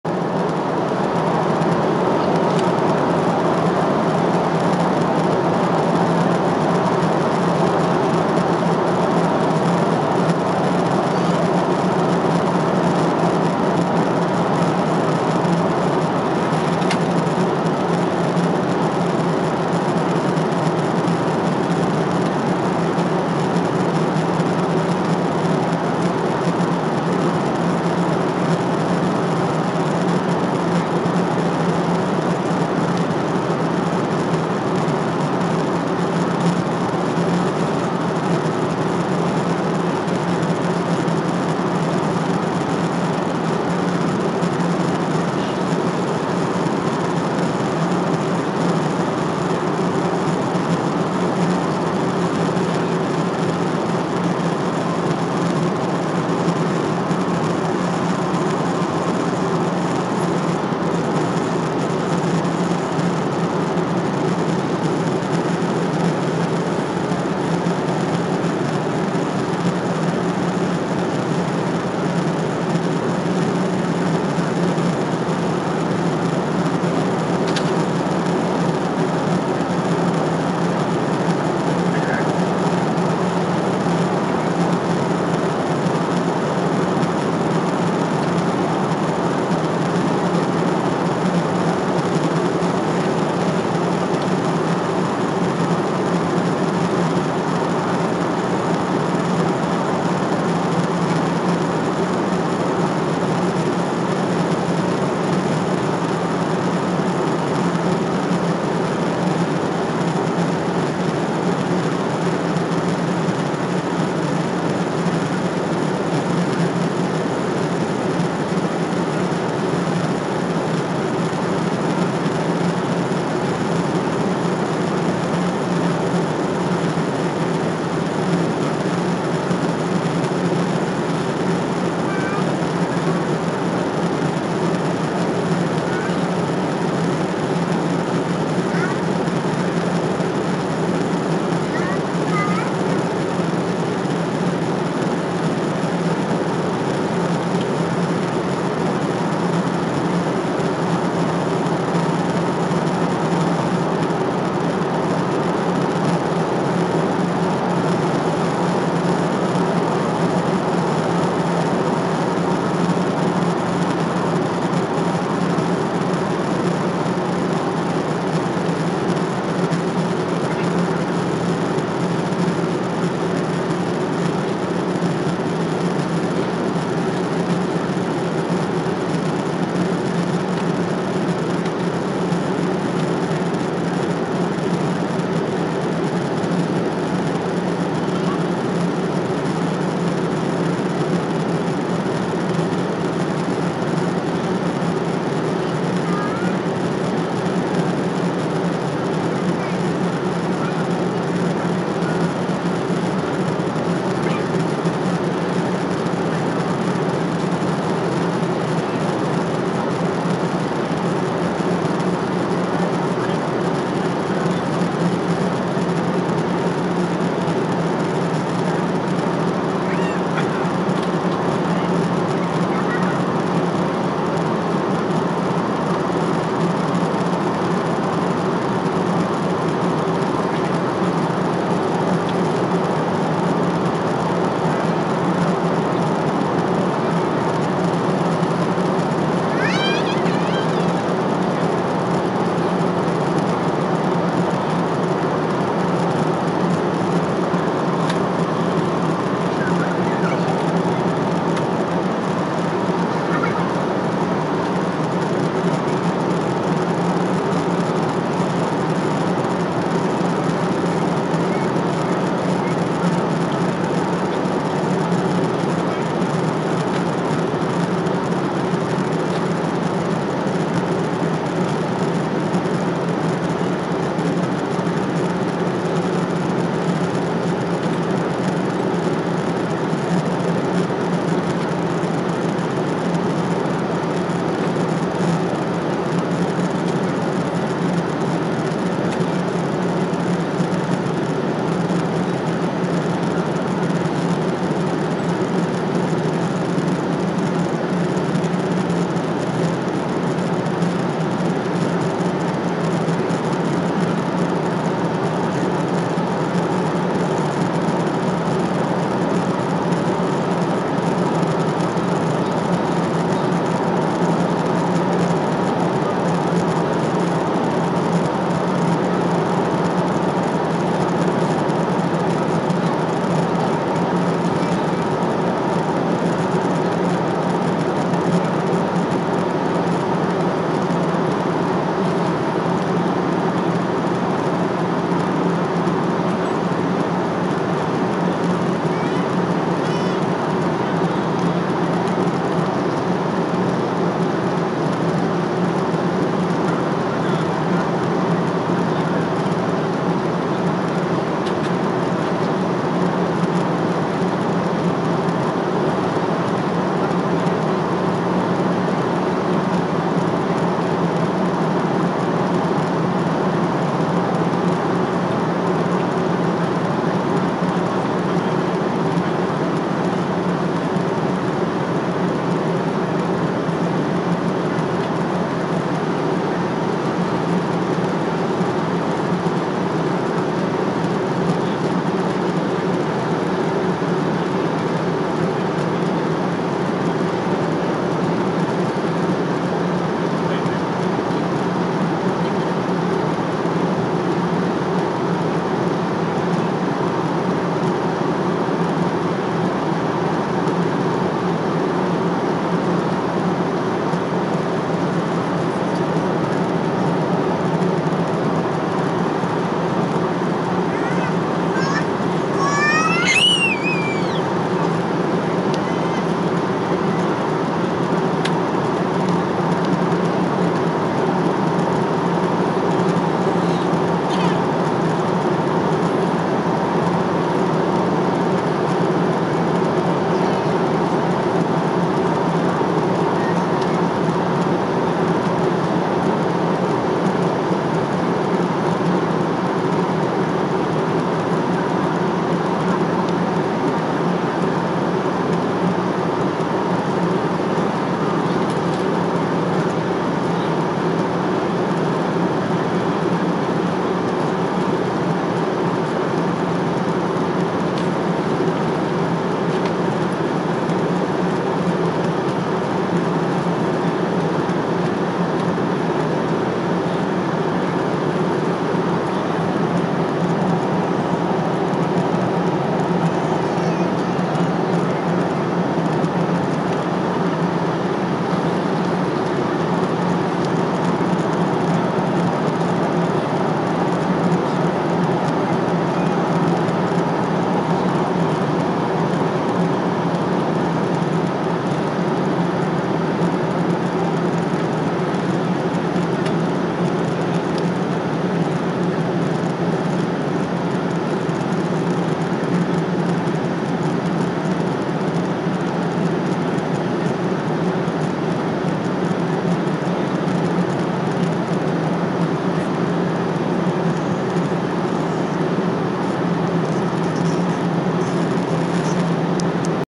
ANA NH173 window view above Alaska #1
アラスカ上空を通過するANA NH173便の機窓です。